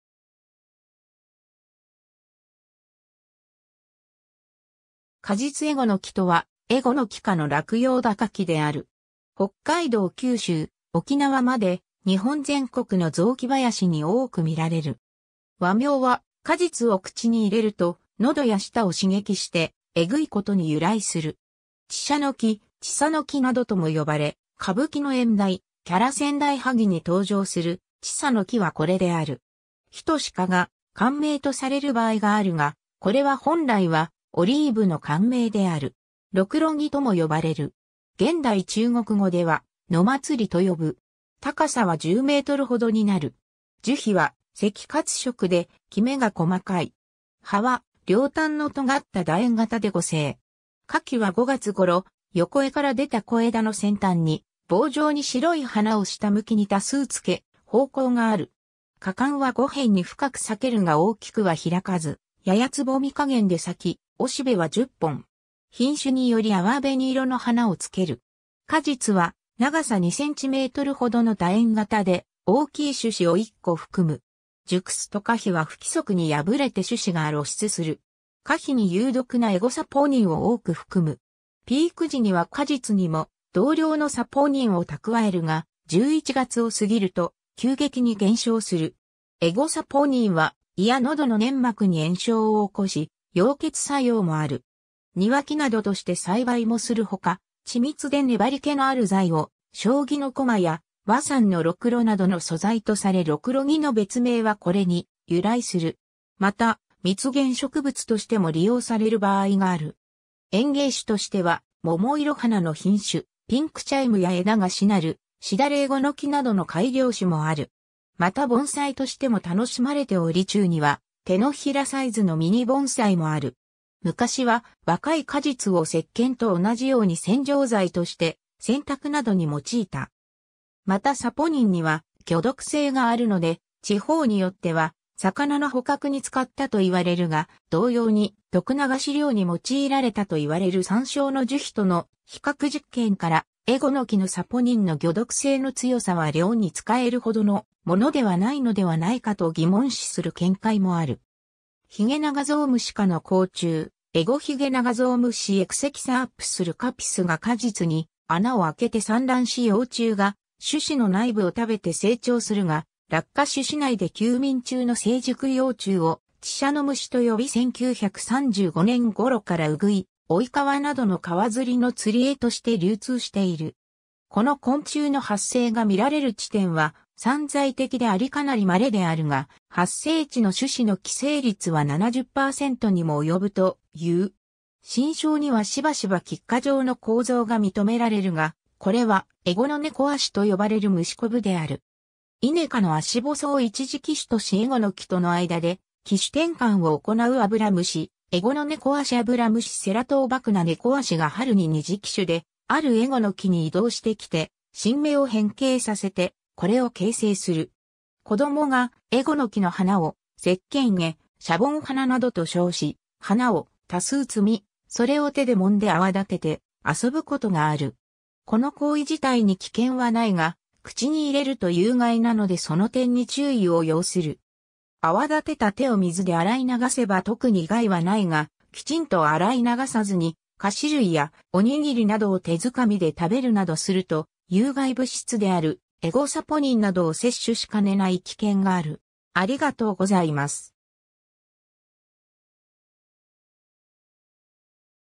エゴノキ
果実 エゴノキ（Styrax japonica）とはエゴノキ科の落葉小高木である。
北海道-九州・沖縄まで、日本全国の雑木林に多く見られる。
 和名は、果実を口に入れると喉や舌を刺激してえぐい（えごい）ことに由来する。
チシャノキ、チサノキなどとも呼ばれ、歌舞伎の演題『伽羅先代萩』に登場するちさの木（萵苣の木）はこれである。
 「齊墩果」が漢名とされる場合があるが、これは本来はオリーブの漢名である。
ロクロギとも呼ばれる。
現代中国語では「野茉莉」と呼ぶ。
 高さは10mほどになる。
樹皮は赤褐色できめが細かい。
葉は両端のとがった楕円形で互生。
花期は5月頃、横枝から出た小枝の先端に房状に白い花を下向きに多数つけ、芳香がある。
花冠は5片に深く裂けるが大きくは開かずややつぼみ加減で咲き、雄しべは10本。
品種により淡紅色の花をつける。
 果実は長さ2cmほどの楕円形で、大きい種子を1個含む。
熟すと果皮は不規則に破れて種子が露出する。
 果皮に有毒なエゴサポニンを多く含む。
ピーク時には果実にも同量のサポニンを蓄えるが、11月を過ぎると急激に減少する。
エゴサポニンは胃や喉の粘膜に炎症を起こし、溶血作用もある。
 庭木などとして栽培もするほか、緻密で粘り気のある材を将棋のこまや和傘のロクロなどの素材とされロクロギの別名はこれに由来する。
 また、蜜源植物としても利用される場合がある。
 園芸種としては桃色花の品種「ピンクチャイム」や枝がしなる「シダレエゴノキ」などの改良種もある。
また盆栽としても楽しまれており中には手のひらサイズのミニ盆栽もある。
 昔は若い果実を石鹸と同じように洗浄剤として洗濯などに用いた。
またサポニンには魚毒性があるので地方によっては魚の捕獲に使ったといわれるが、同様に毒流し漁に用いられたと言われるサンショウの樹皮との比較実験からエゴノキのサポニンの魚毒性の強さは漁に使えるほどのものではないのではないかと疑問視する見解もある。
 ヒゲナガゾウムシ科の甲虫・エゴヒゲナガゾウムシ（ウシヅラカミキリ） Exechesops leucopis（Jordan, 1928）が果実に穴を開けて産卵し幼虫が種子の内部を食べて成長するが落下種子内で休眠中の成熟幼虫を「ちしゃの虫」と呼び1935年ごろからウグイ、オイカワなどの川釣りの釣り餌として流通している。
この昆虫の発生が見られる地点は散在的でありかなり稀であるが、発生地の種子の寄生率は70%にも及ぶという。
 新梢にはしばしば菊花状の構造が認められるが、これはエゴノネコアシと呼ばれる虫こぶである。
イネ科のアシボソを一次寄主としエゴノキとの間で寄主転換を行うアブラムシ、エゴノネコアシアブラムシ Ceratovacuna nekoashi（Sasaki, 1907）が春に二次寄主であるエゴノキに移動してきて新芽を変形させてこれを形成する。
 子供が、エゴノキの花を「セッケン花」「シャボン花」などと称し、花を多数摘み、それを手で揉んで泡立てて遊ぶことがある。
この行為自体に危険はないが、口に入れると有害なのでその点に注意を要する。
泡立てた手を水で洗い流せば特に害は無いが、きちんと洗い流さずに菓子類やオニギリなどを手づかみで食べるなどすると有害物質であるエゴサポニンなどを摂取しかねない危険がある。